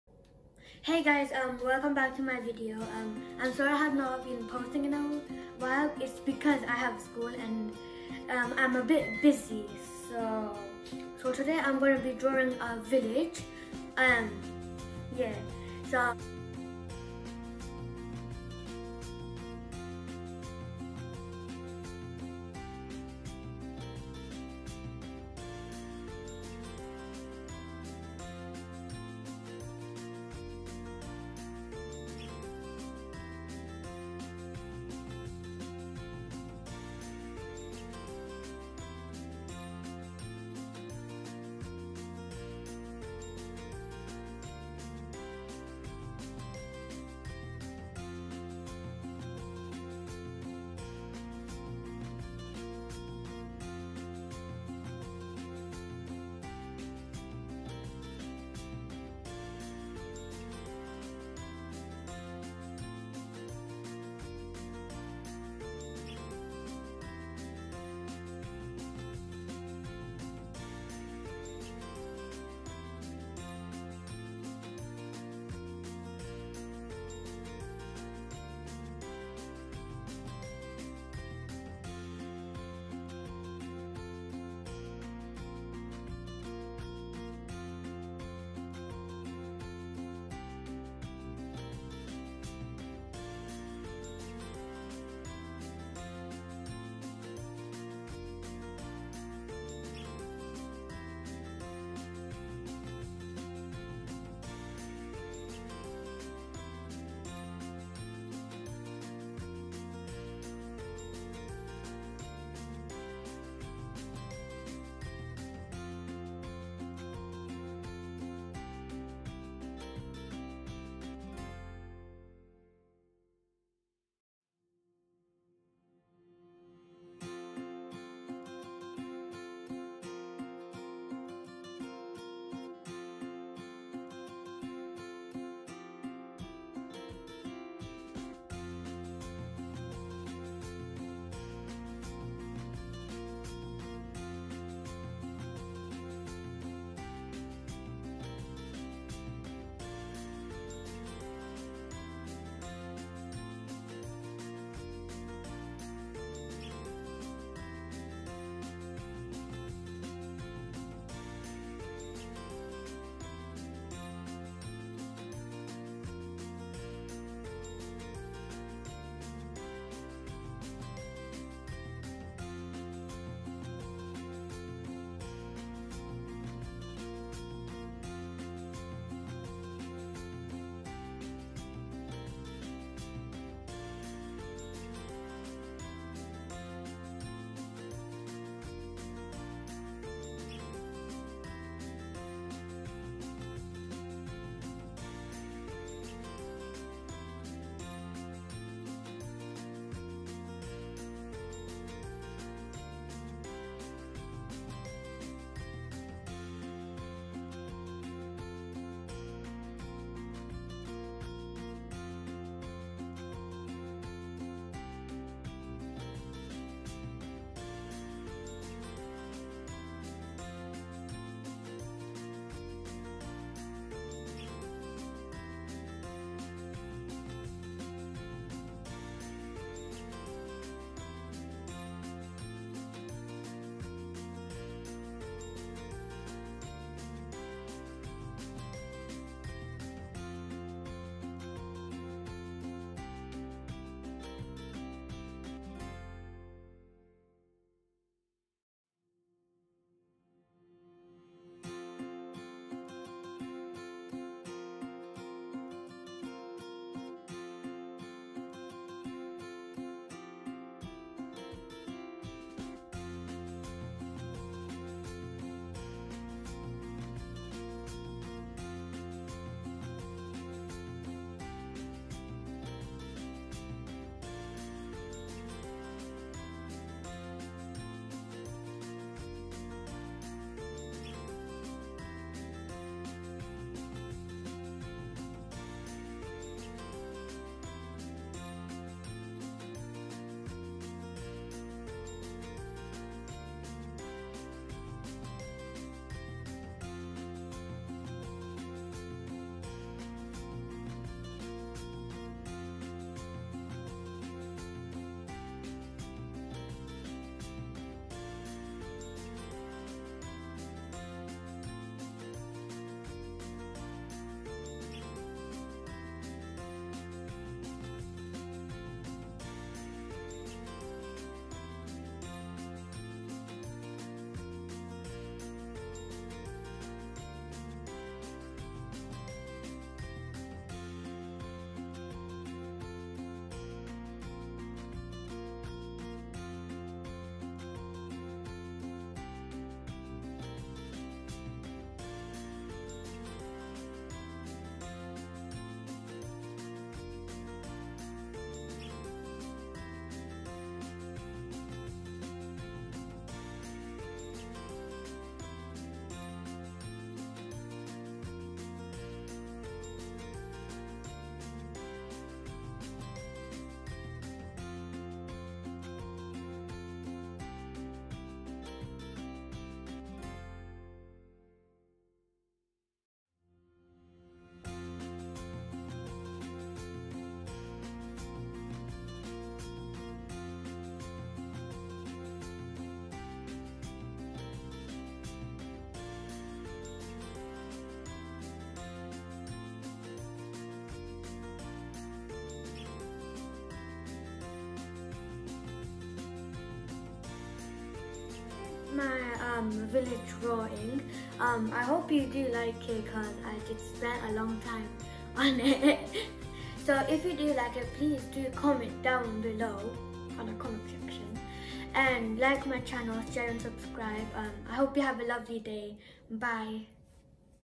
A VILLAGE 🏡🏡
I hope everyone will enjoy this drawing
I know I am not that much  good at drawing but
I love it 🥰 I just like to draw😎😎😎